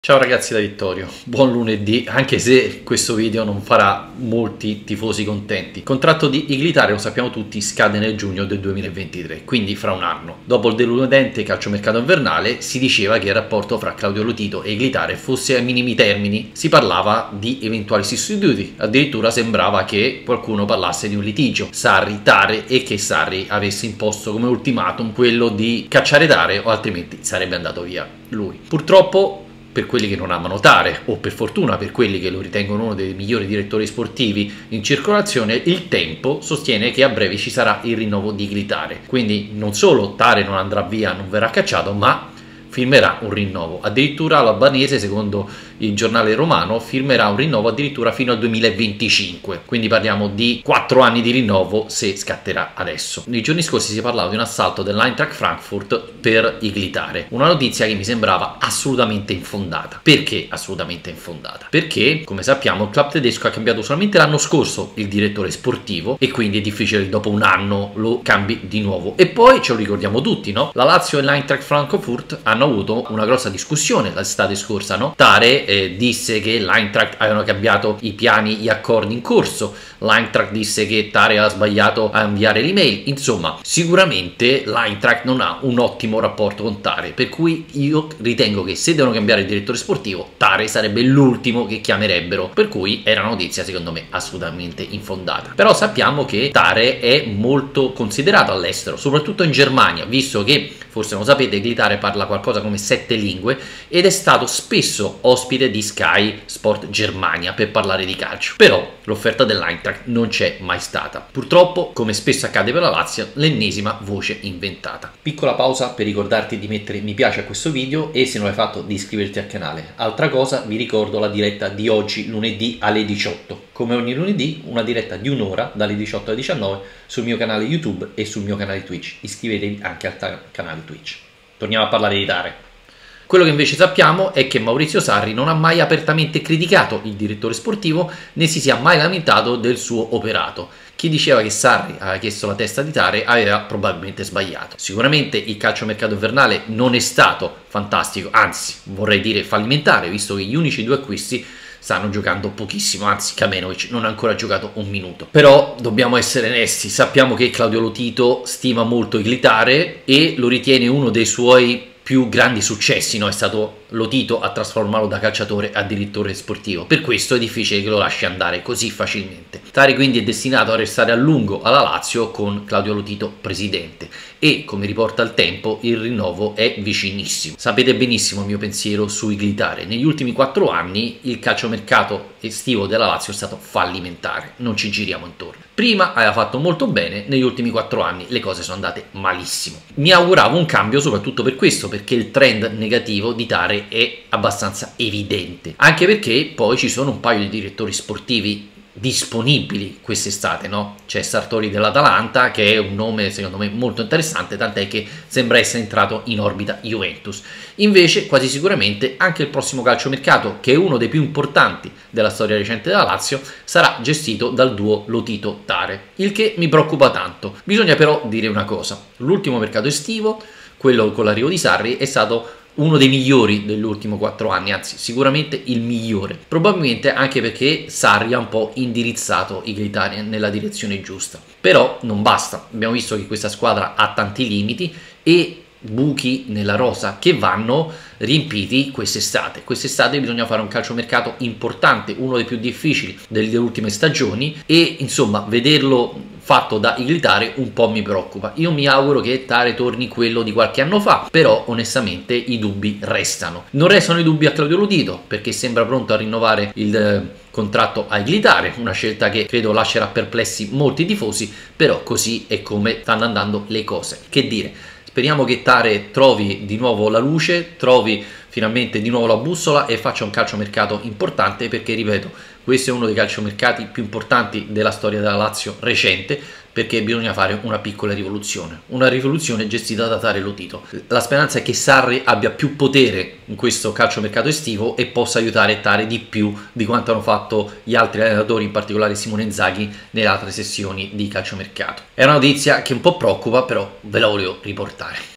Calciomercato Lazio, Igli Tare rinnova?
Il contratto di Igli Tare scade nel giugno del 2023, quindi fra un anno. Dopo il deludente calciomercato invernale si diceva che il rapporto tra Claudio Lotito e Igli Tare fosse ai minimi termini.